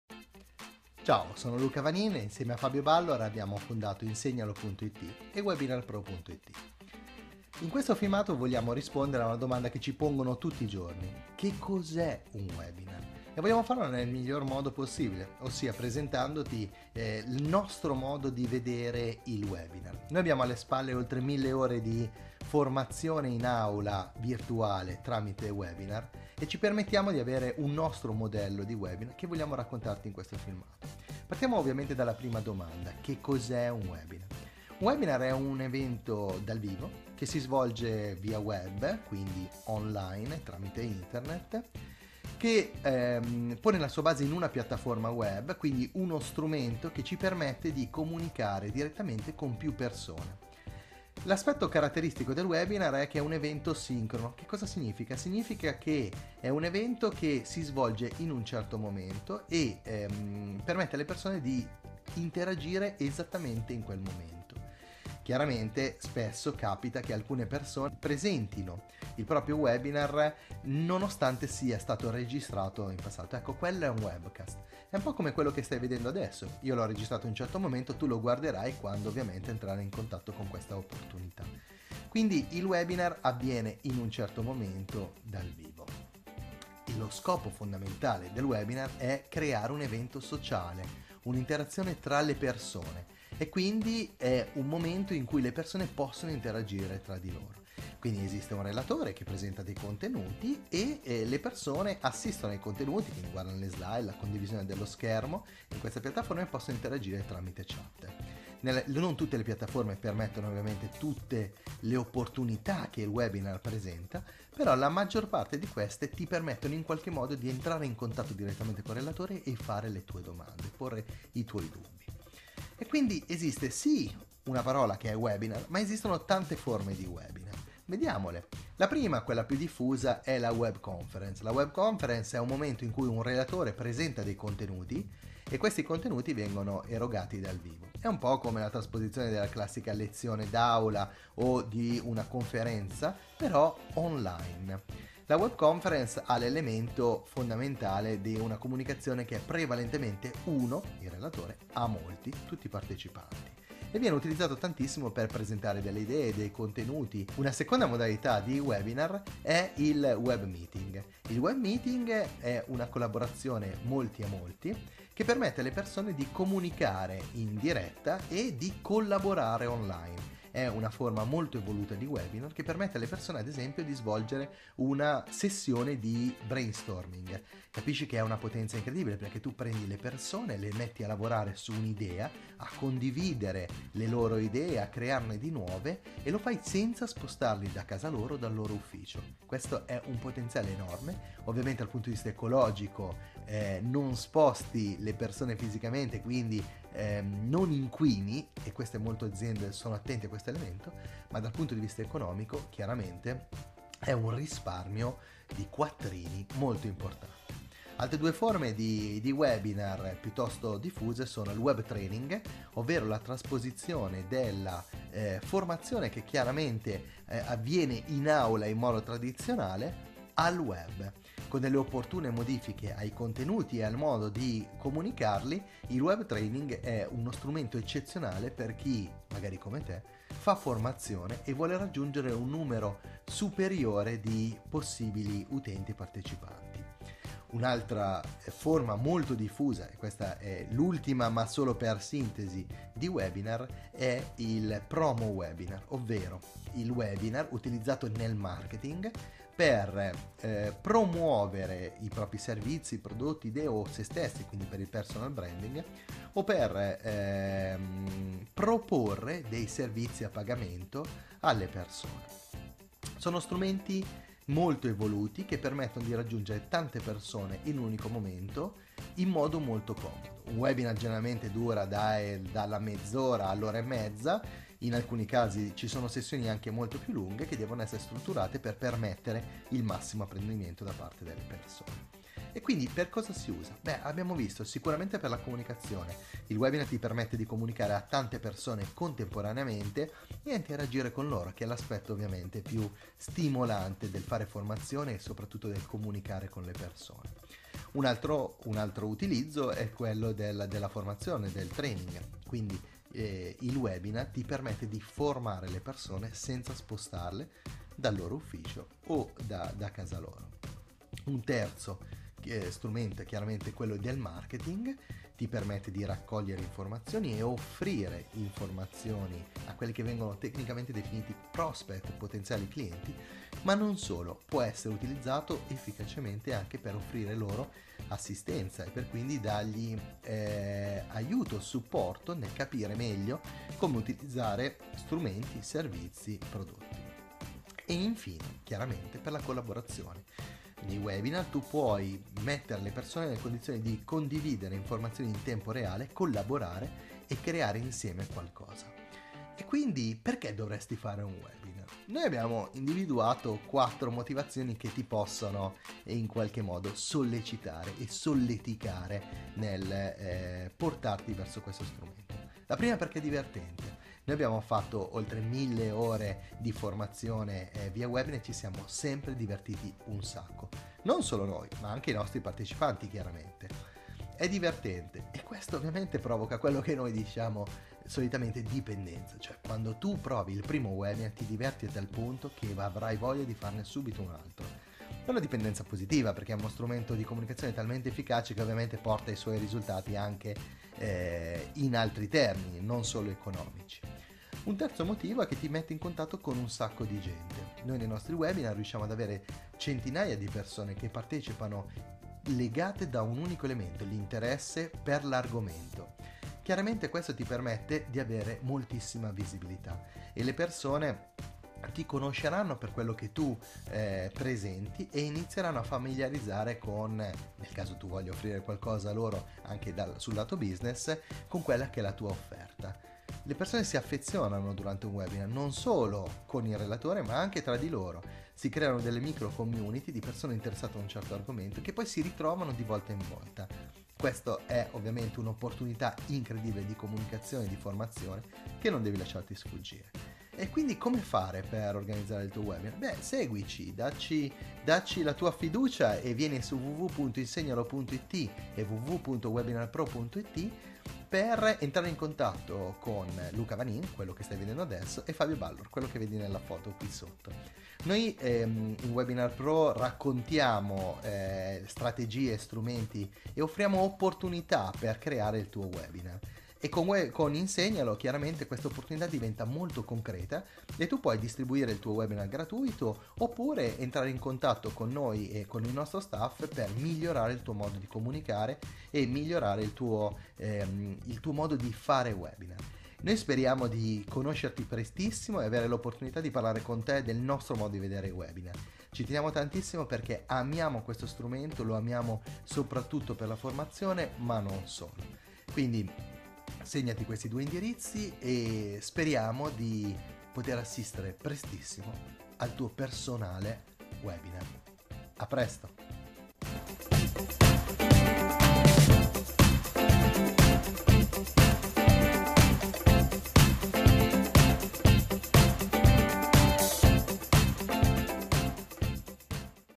Cosa è un Webinar, come funziona e come puoi usarlo
Cos'è e a cosa serve un webinar?
Ci sono molti modi di chiamarlo: videoconferenza, webmeeting, webconference ma tutti indicano la stessa cosa. In questi pochi minuti capirai meglio di cosa si tratta!
Se stai guardando questo video perchè devi realizzare un webinar, la soluzione migliore è andare su (siamo sempre noi!)